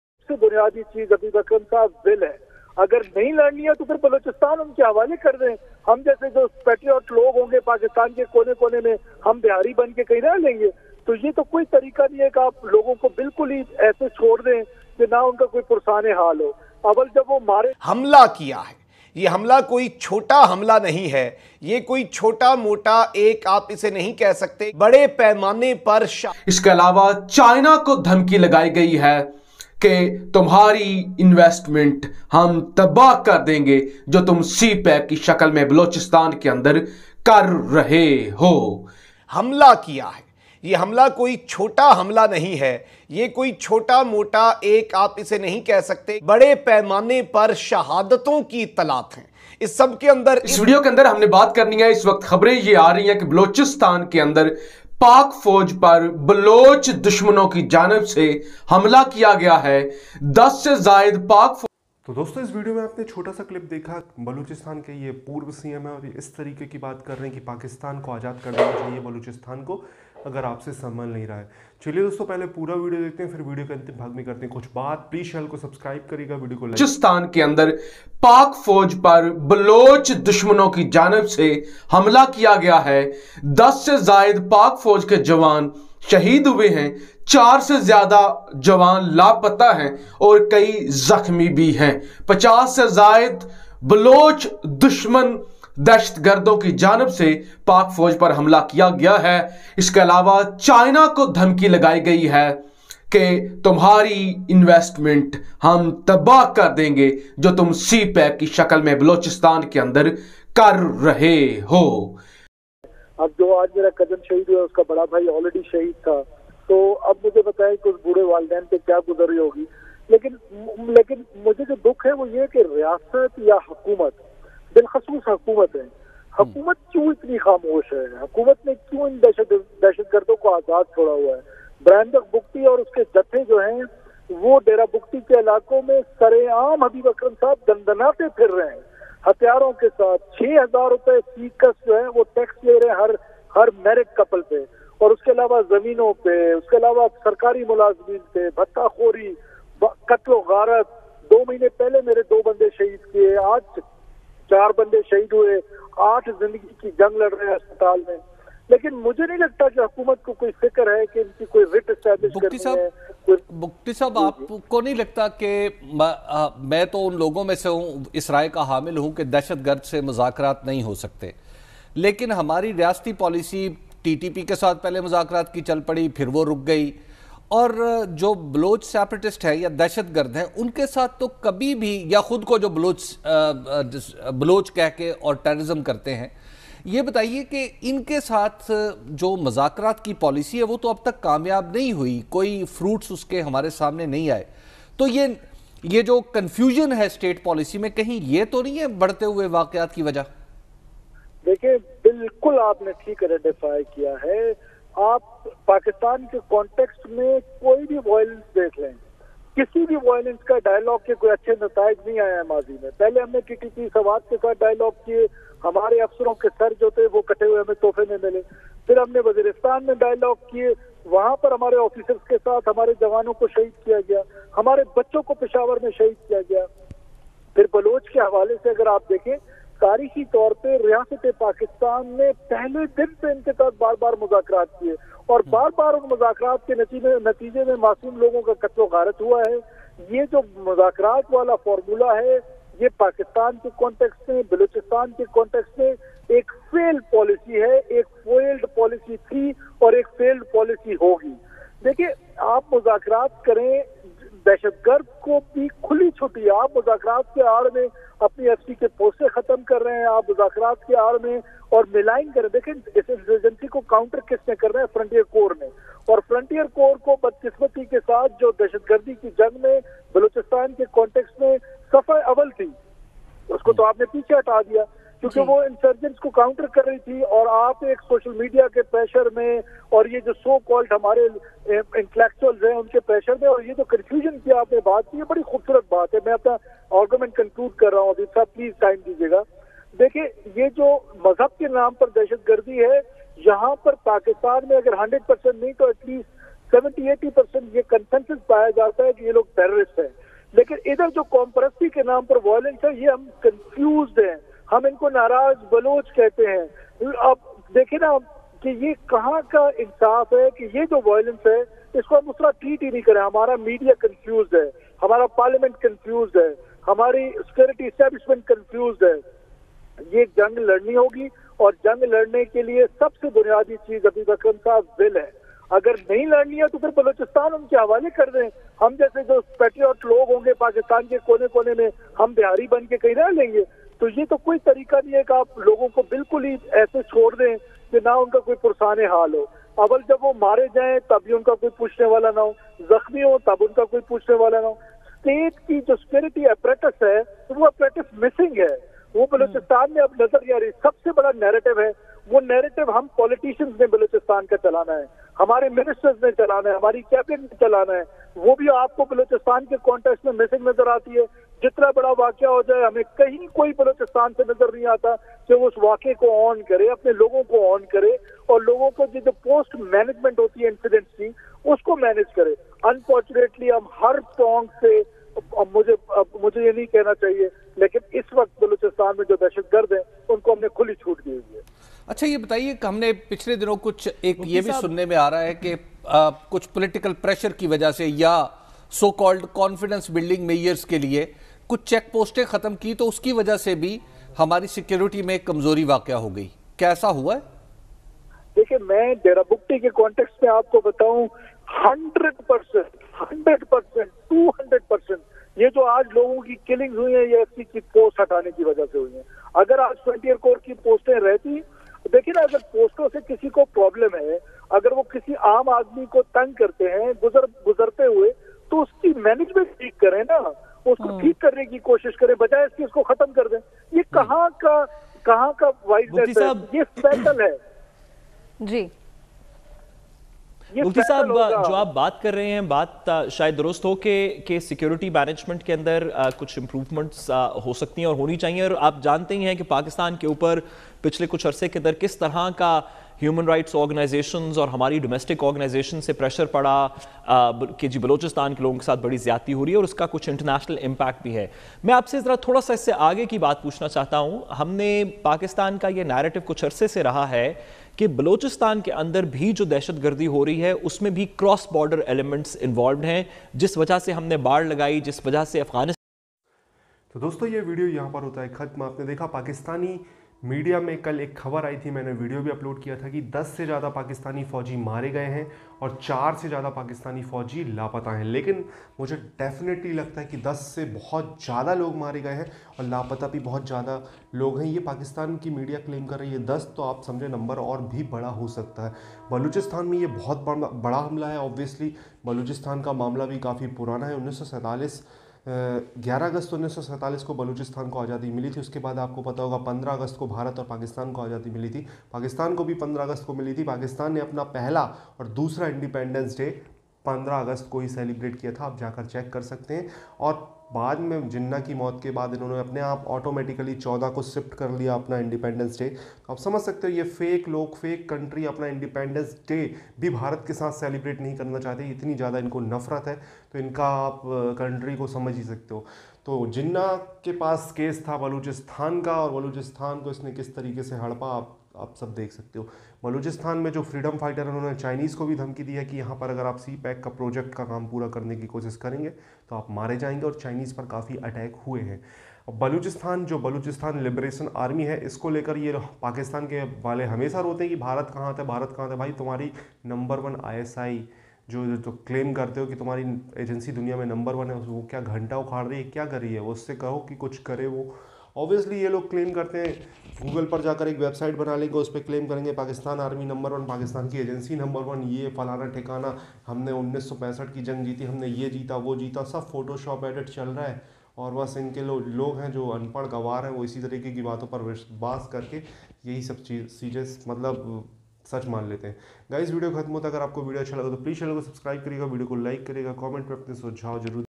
बलूचिस्तान से भाग रहे हैं पाकिस्तानी फौजी | Pak Media latest on India
Hassan nisar latest reaction on India
 Rauf klasra latest reaction on India